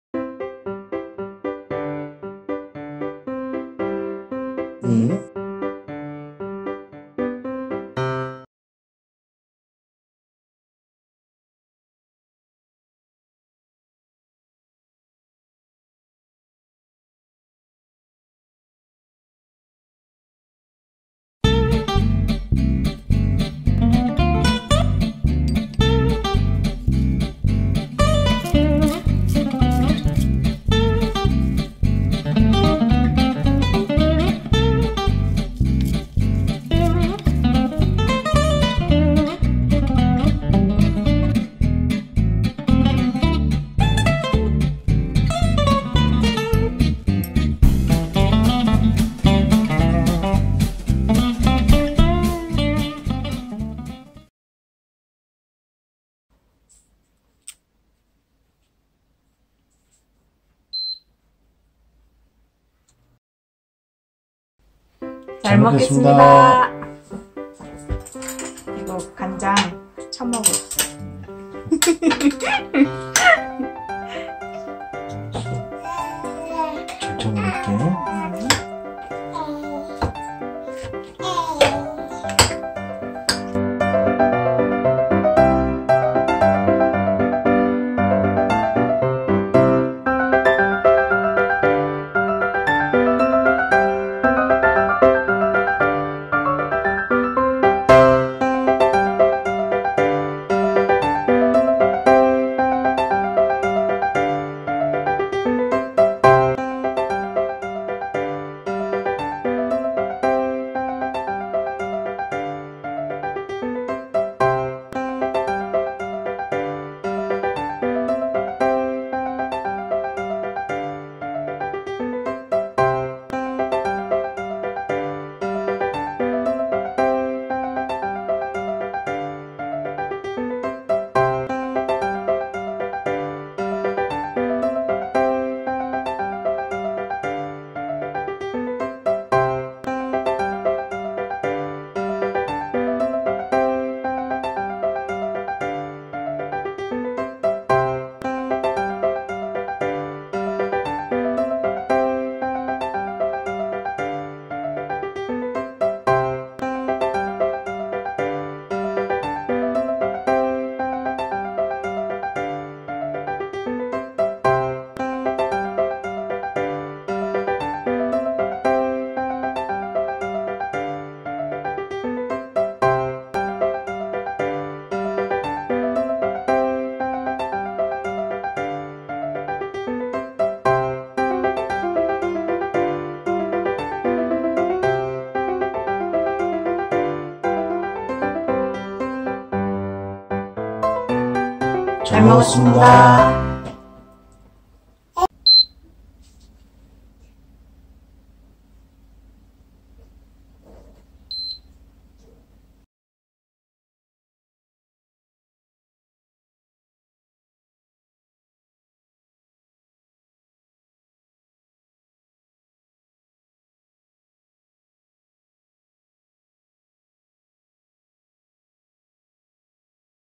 [당뇨생활] 몸이 좋아하는 야채비빔밥! 혈당에는 어떨까? (당뇨에 좋은 음식:당뇨식단)
당화혈색소 11.6(공복혈당 306)이었던 남편 야수와 그의 아내 마녀가 이런저런 음식을 먹으며 혈당을 체크해보는 채널입니다.

오늘은 야채비빔밥을먹고 혈당을 측정해보겠습니다 :)